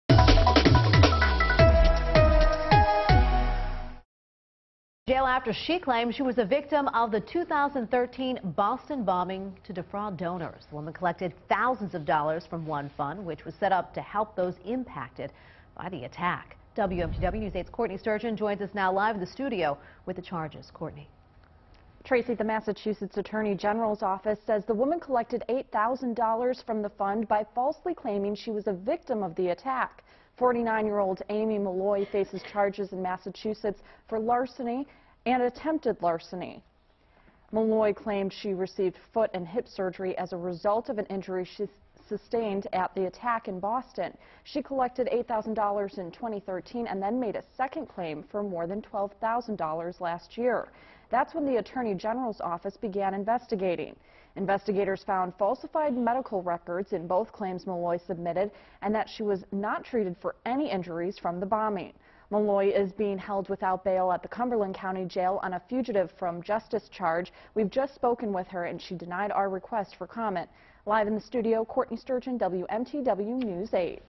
Portland woman faces Massachusetts fraud charge
A Portland woman was arrested Wednesday on charges of defrauding The One Fund, which was set up to help victims following the 2013 Boston Marathon bombing.